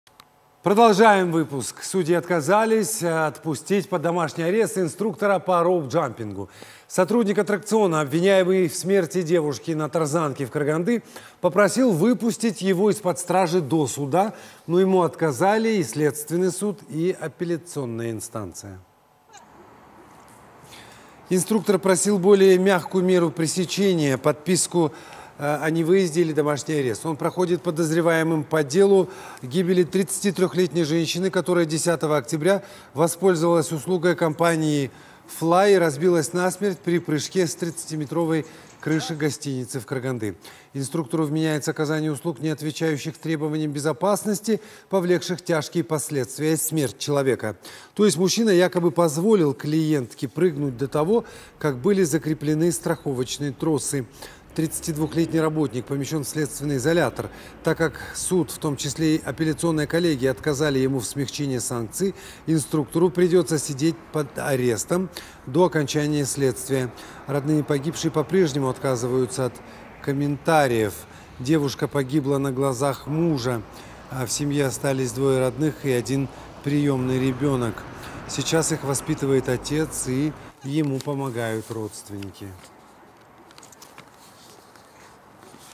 Инструктора, обвиняемого в гибели женщины на тарзанке, не отпустили под домашний арест
Судьи отказались отпустить под домашний арест инструктора по роуп-джампингу. Сотрудник аттракциона, обвиняемый в смерти девушки на тарзанке в Караганде, попросил выпустить его из-под стражи до суда, но ему отказали и следственный суд, и апелляционная инстанция.
Он проходит подозреваемым по делу о гибели 33-летней женщины, которая 10 октября воспользовалась услугой компании «Флай» и разбилась насмерть при прыжке с 30-метровой крыши гостиницы в Караганде. Инструктору вменяется «Оказание услуг, не отвечающих требованиям безопасности, повлекших тяжкие последствия – смерть человека». То есть, мужчина якобы позволил клиентке прыгнуть до того, как были закреплены страховочные тросы. 32-летний работник помещен в следственный изолятор. Так как суд, в том числе апелляционная коллегия, отказали ему в смягчении санкции, инструктору придется сидеть под арестом до окончания следствия. Родные погибшей по-прежнему отказываются от комментариев. Девушка погибла на глазах мужа. В семье остались двое родных и один приемный ребенок. Сейчас их воспитывает отец, ему помогают родственники.